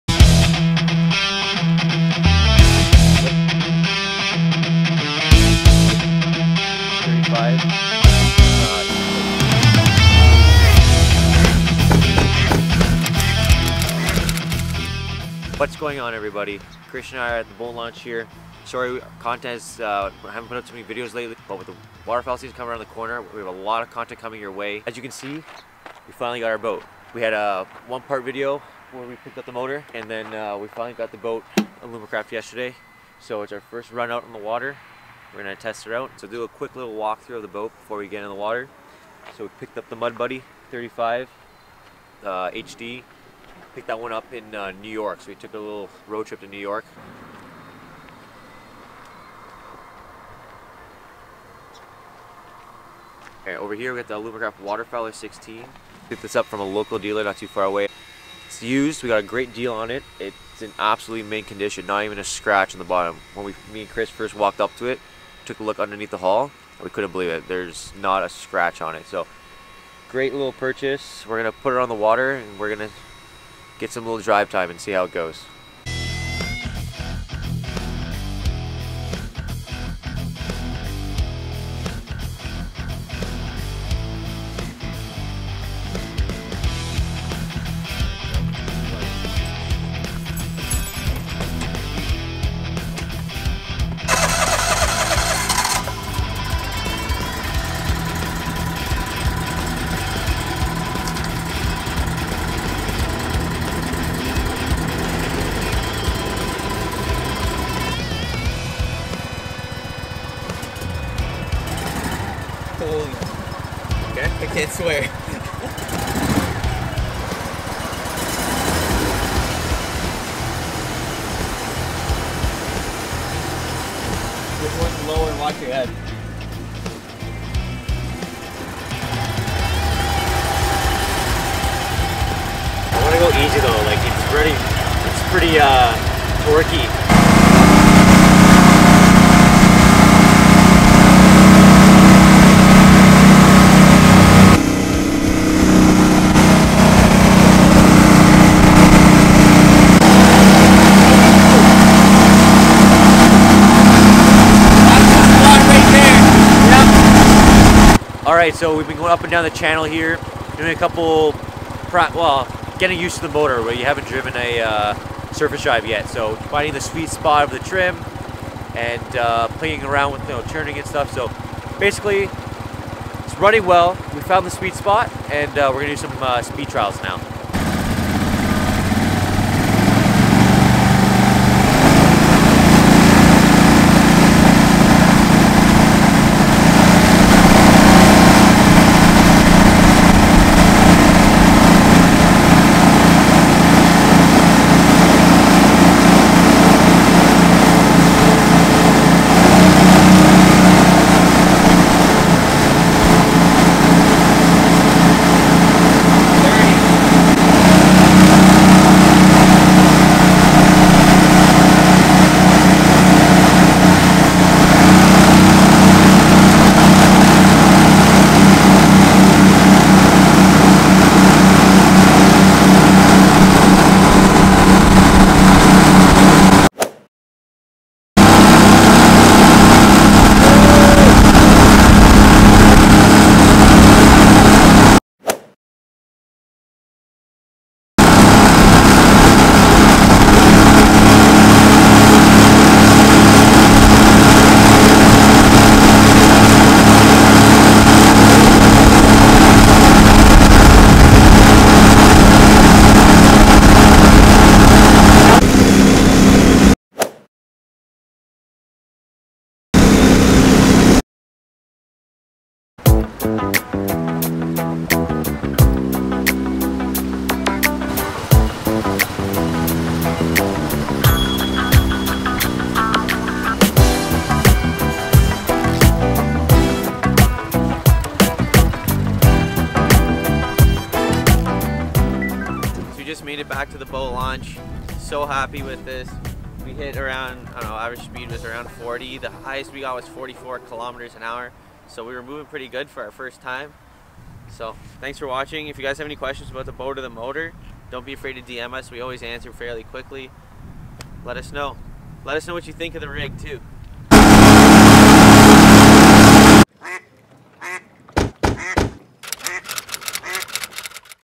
New Boat! Mud Buddy 35HD on Alumacraft Waterfowler 1648
Watch as the GRF crew takes the new duck boat on the river for its inaugural run. Mud Buddy 35HD sitting on a Alumacraft waterfowler 16. What rig are you running?